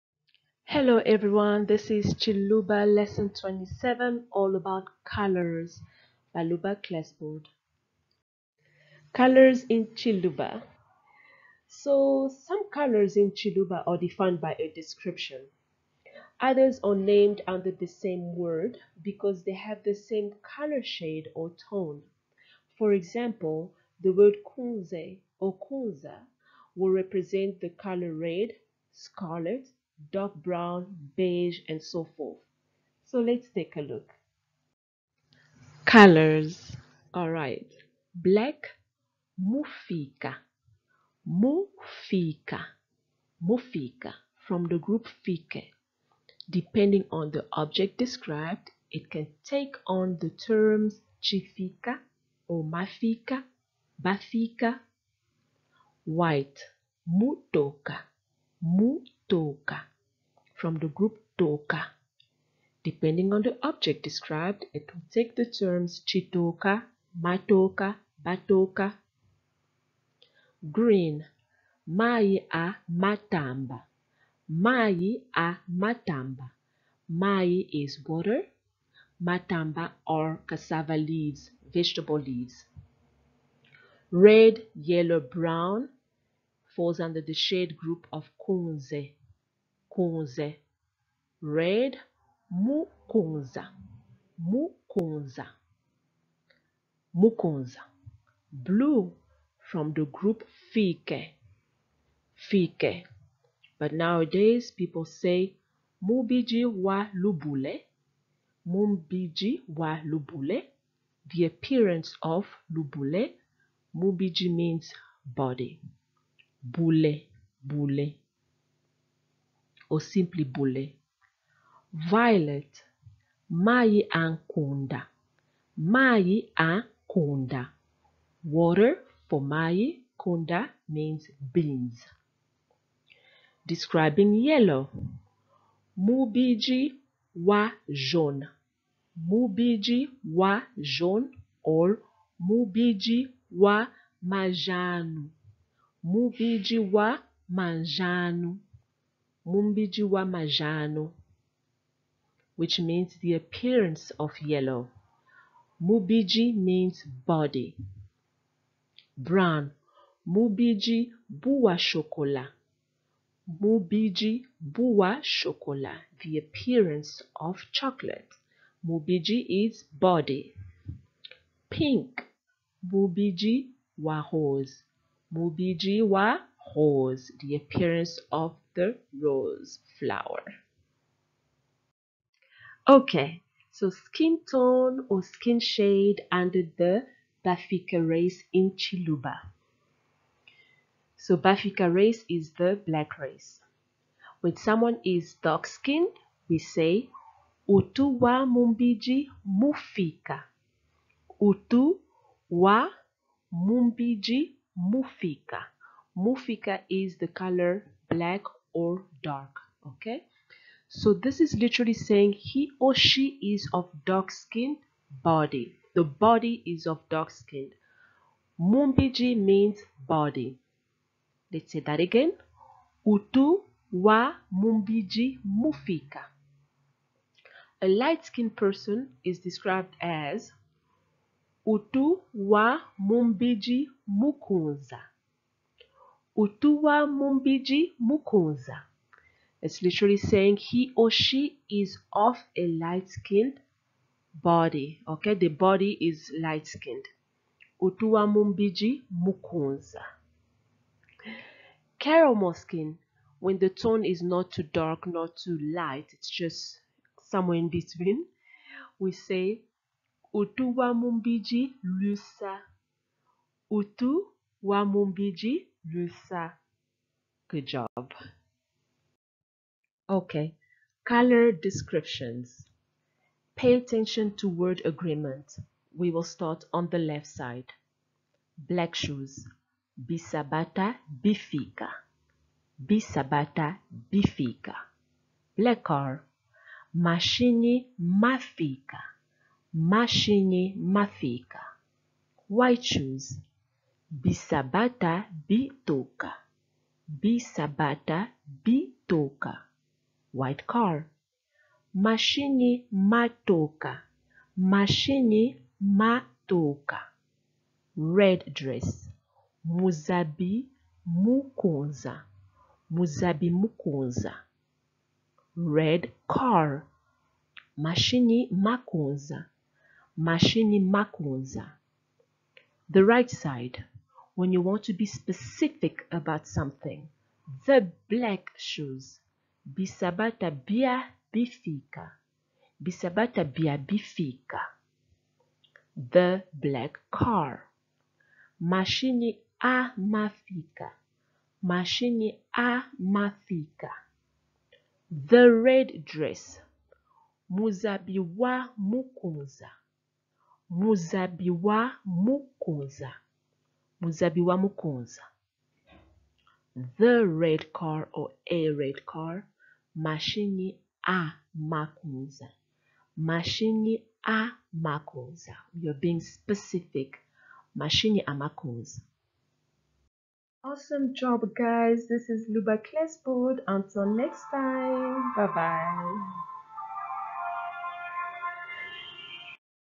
Lesson 27: Colors_Color Groups
Learn all about colors/colours in Tshiluba language. The grouping of words and descriptions defines a color shade. Per Tshiluba grammar, colors and objects have to agree in quantity (Lesson 12 Word Agreement).
Some sub-Luba dialects have different terms for color.